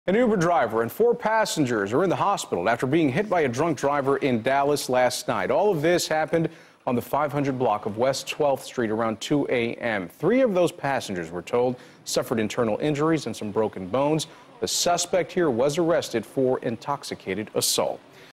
Suspected Drunk Driver Crashes Into Uber With 4 Passengers
Dallas police say a suspected drunk driver crashed into an Uber early Sunday morning, sending the Uber driver and four passengers to the hospital.